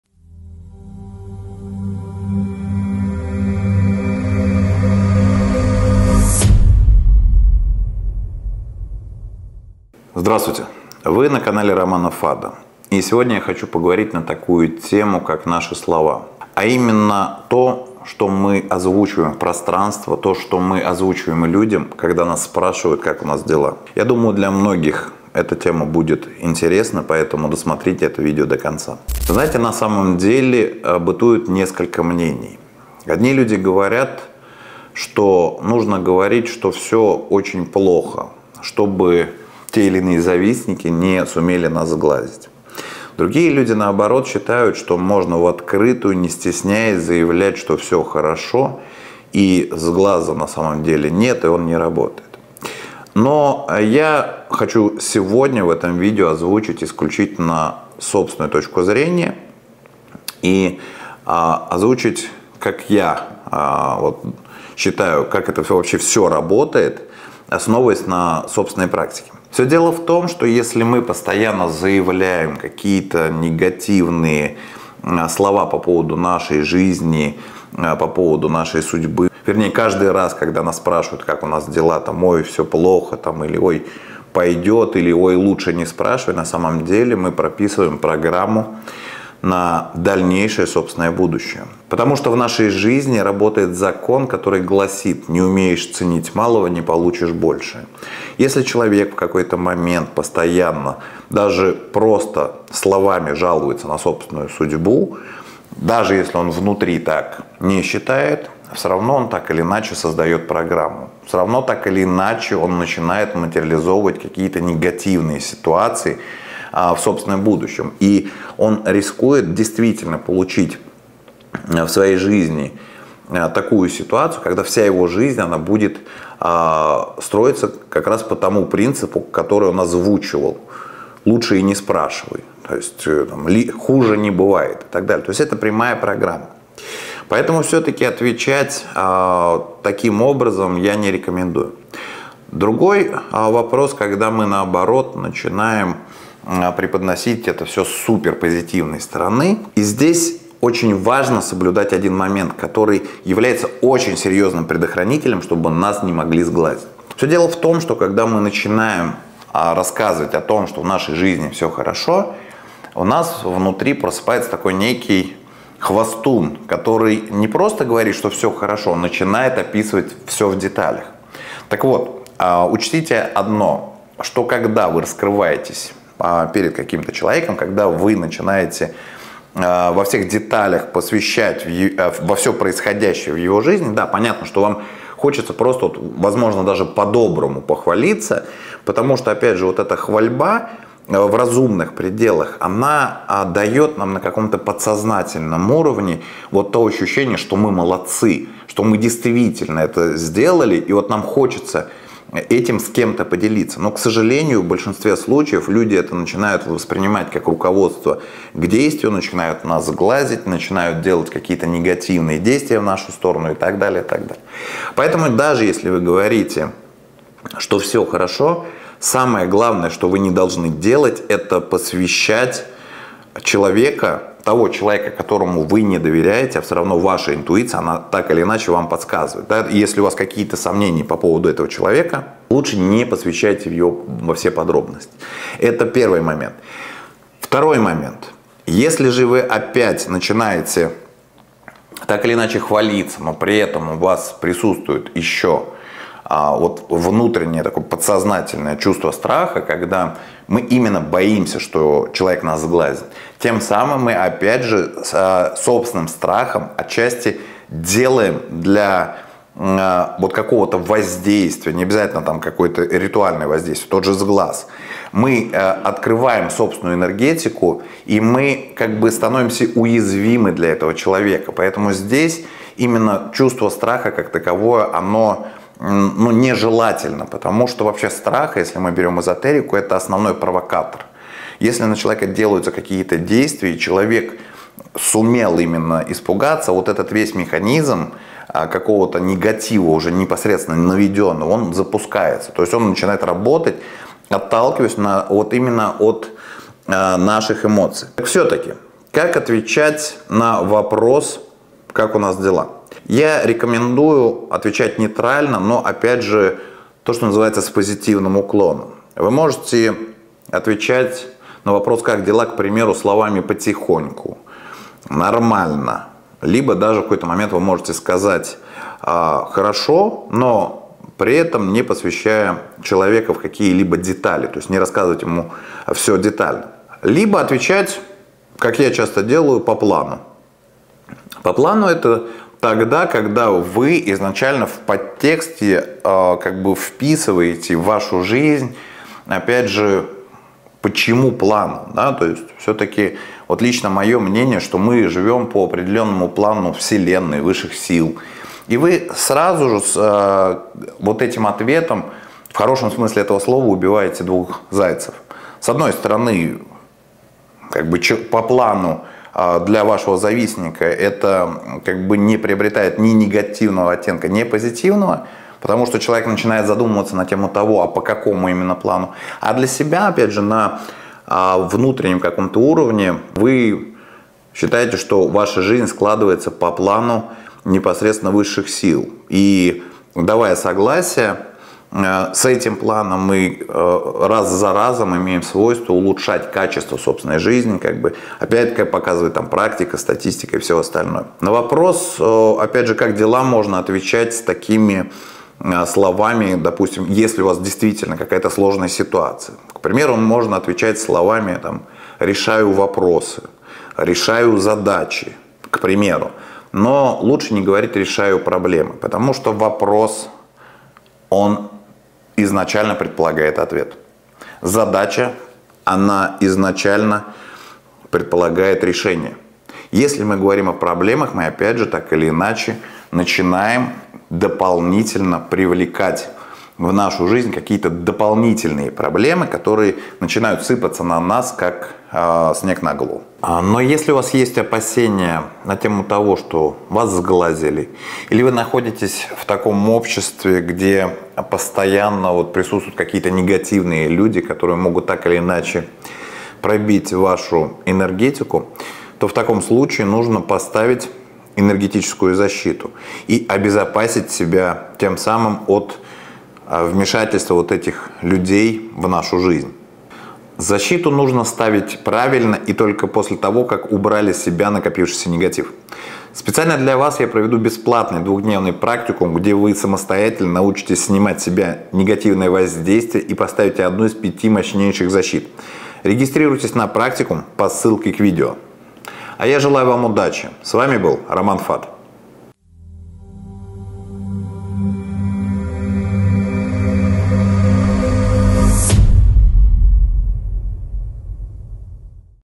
Никогда не отвечайте на вопрос «как дела?» этими словами
В этом видео вы узнаете:

— Влияние наших слов на нашу реальность

 — Как другие люди могут вас сглазить, когда вы  делитесь своими успехами?

— Как отвечать на вопрос «как дела?»